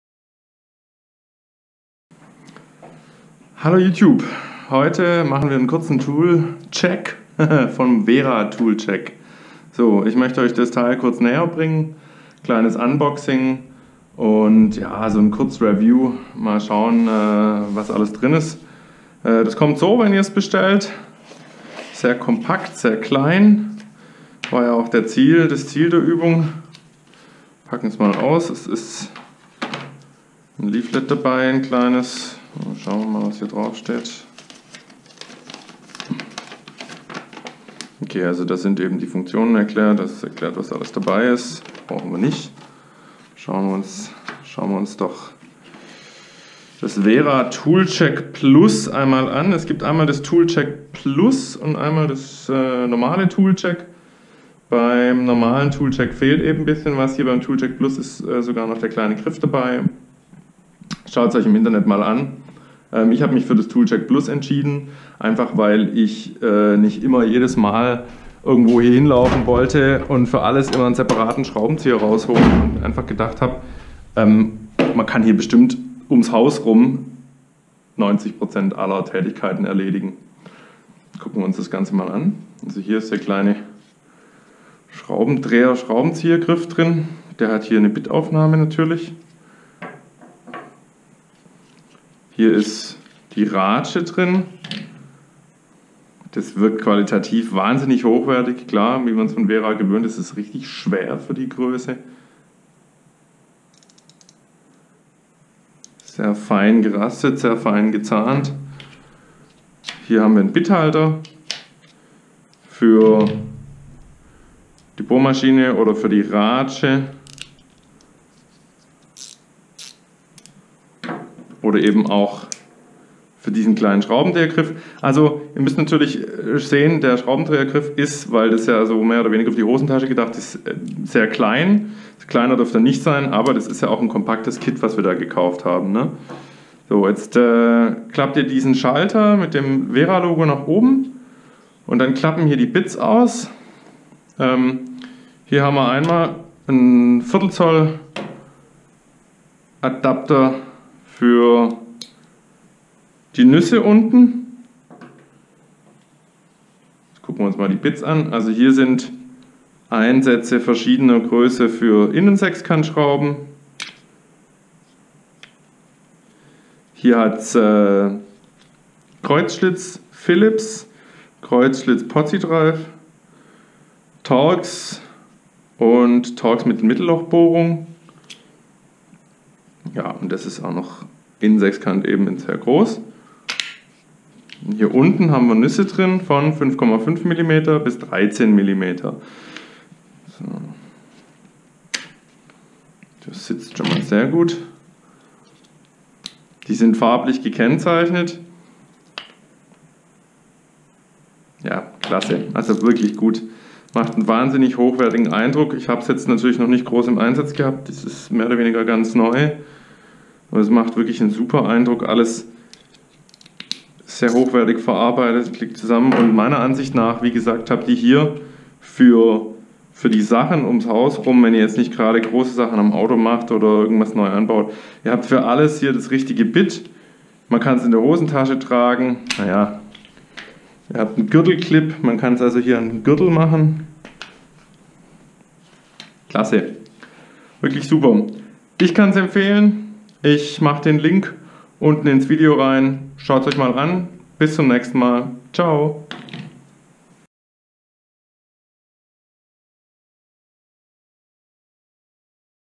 WERA Tool-Check: Ein kompletter Werkzeugkoffer am Gürtel? Kurzvorstellung.
Eine kurze Vorstellung des WERA Tool-Check, welches ich mir gekauft habe, um nicht für jede kleine Aufgabe den ganzen Werkzeugkoffer mitbringen zu müssen. Das Bitsortiment umfasst eine große Sammlung an wichtigen Bits: Kreutzschlitz (Pozidriv und Philips), Torx, Torx mit Mittenloch, sowie Bits für Schrauben mit Innensechskant. Ein Schlitz-Einsatz ist auch dabei. Verschiedene Steckschlüsseleinsätze, umgangsprachlich als "Nuss" bezeichnet, runden das Ganze ab.
Im Video stelle ich das Bitsortiment mit Ratsche und Schraubendreher-Griff kurz vor.

Link zum Produkt: